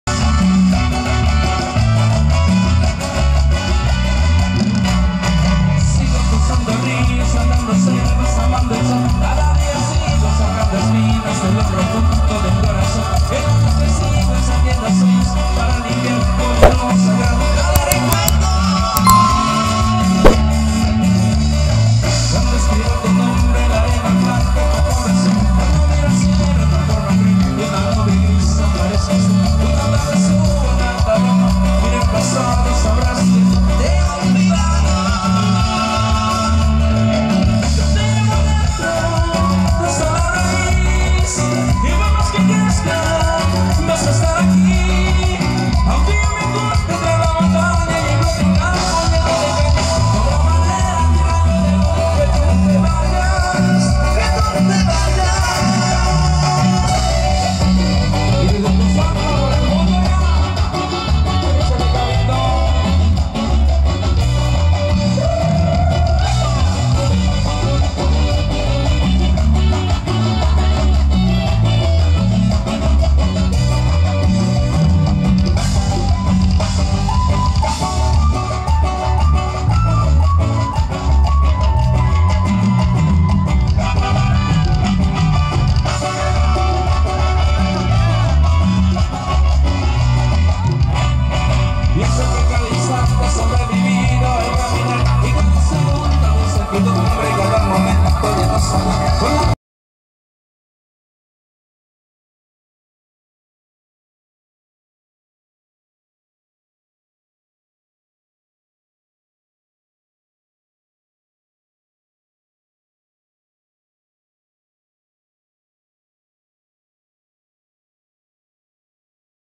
Esta CHICA de ROJO lucio muy DIVINA en este lindo zapateado CALENTANO 😍
Amigos aqui les dejamos este bailazo en Cutzamala, Gro.💃🎀🔥😍📹